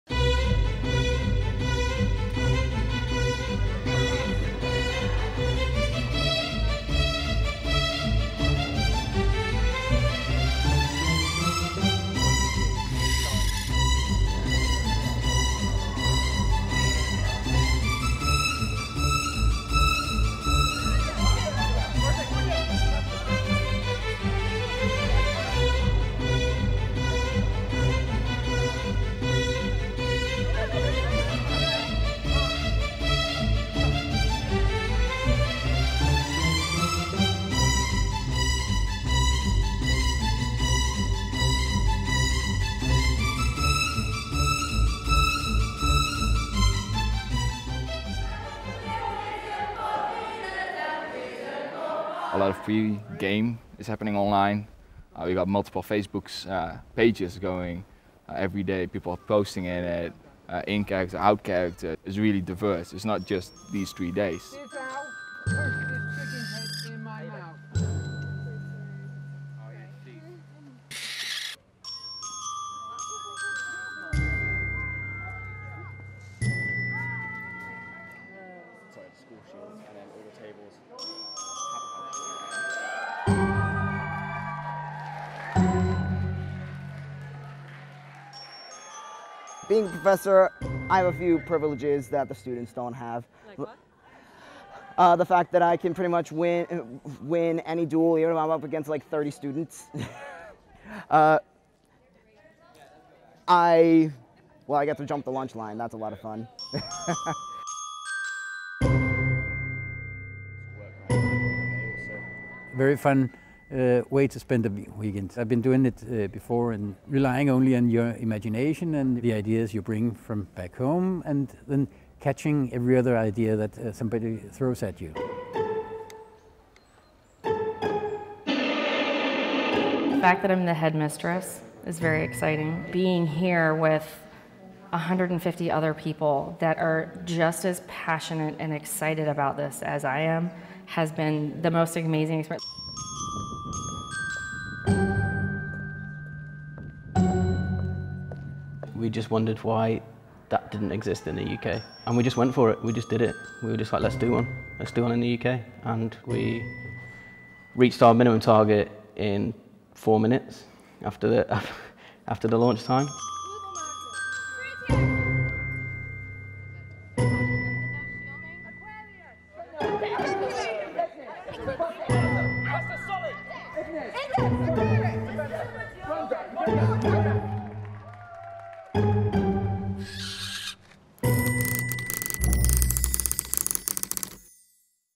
I went to a Harry Potter-inspired school and this is what I learned
The Bothwell School of Witchcraft in East Sussex welcomes role-players who want to impersonate witches in a Harry Potter-inspired setting.